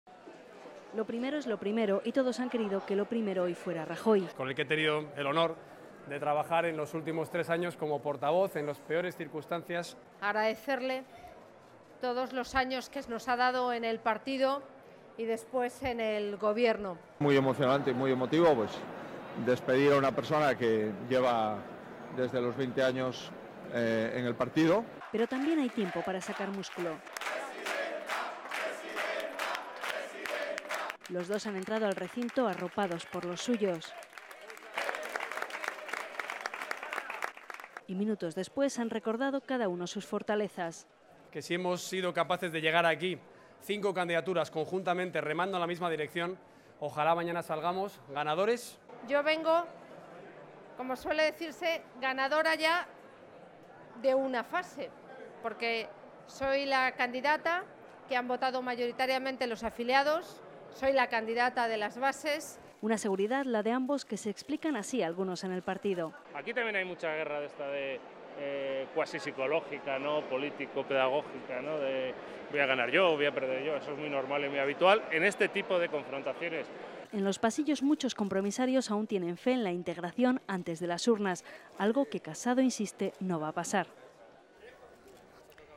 Casado o Santamaría: los dos están seguros de su victoria
Rajoy deja la primera línea de la política y por primera vez en un congreso de los populares, no se conoce todavía quién será su sucesor. La decisión está en manos de los compromisarios, los más perseguidos el primer día de congreso.